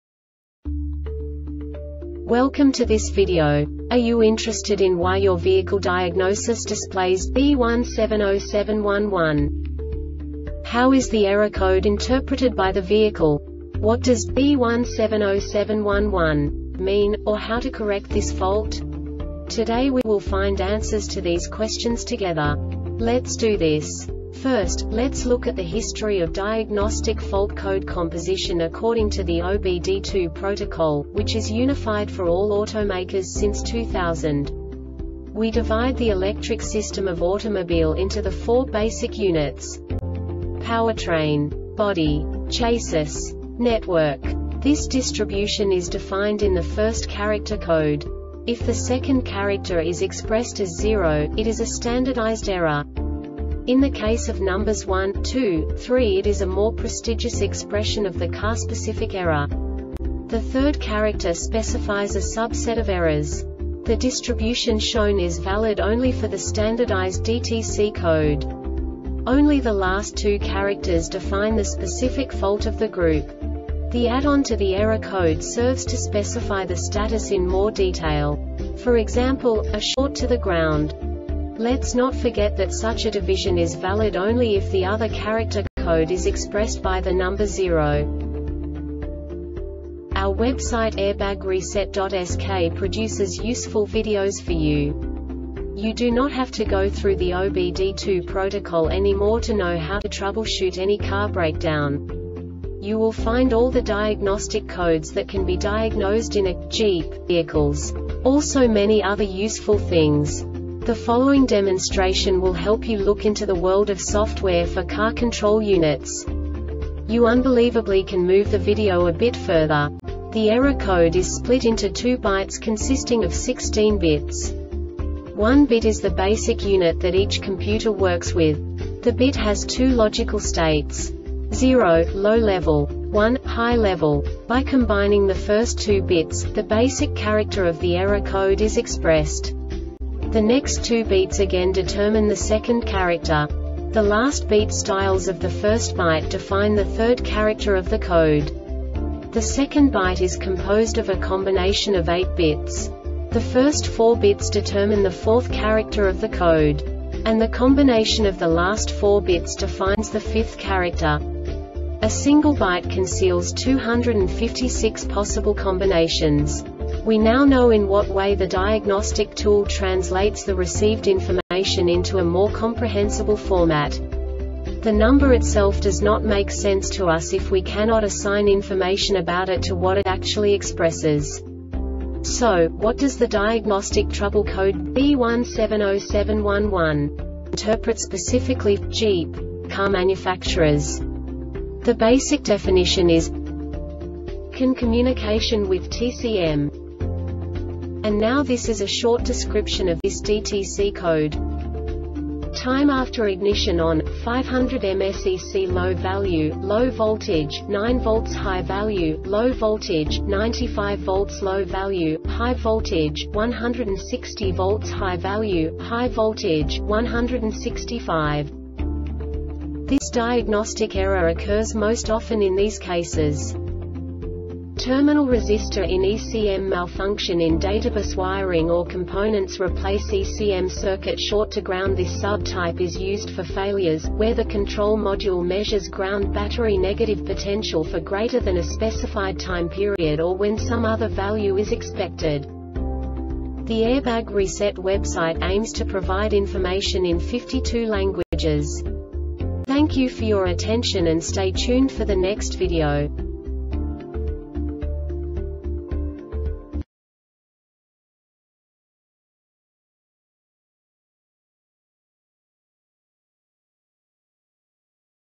DTC Jeep B1707-11 Short Explanation
Contents:
0:21 Basic DTC analysis according to OBD2 protocol standard.
1:48 Insight into programming
2:58 A diagnostically understandable description of the Diagnostic Trouble Code
3:05 Basic error definition

Make:
Jeep
Code:
B1707 11
Definition:
CAN communication with TCM
Description: